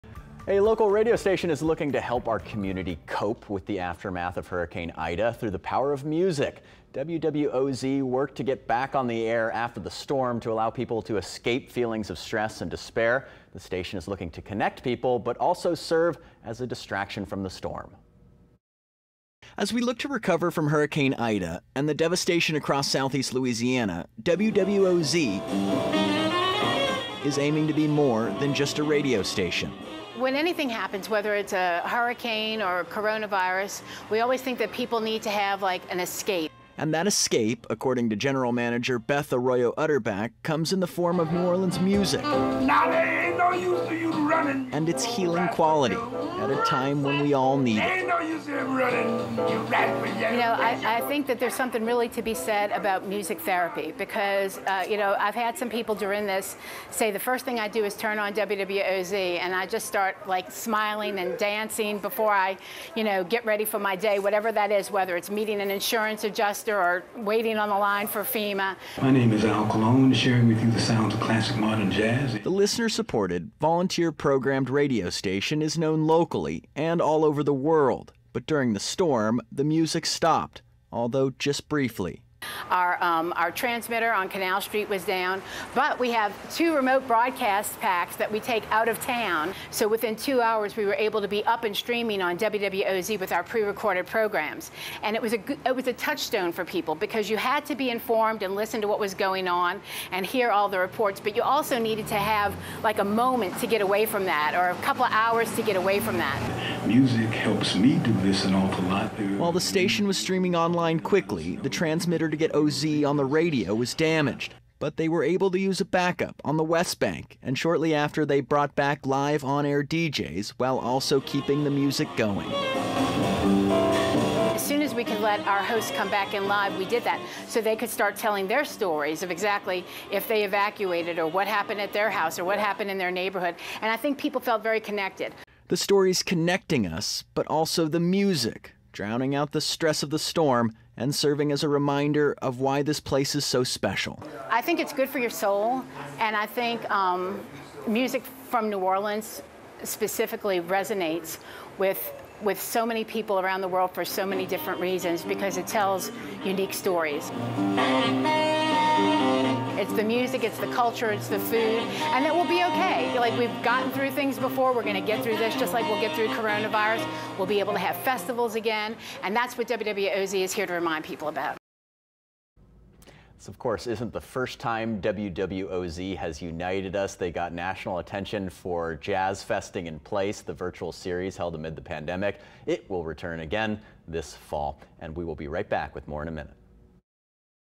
WWOZ helping community cope post-Ida
WWOZ worked to get back on air after the storm to allow people to escape feelings of stress and despair.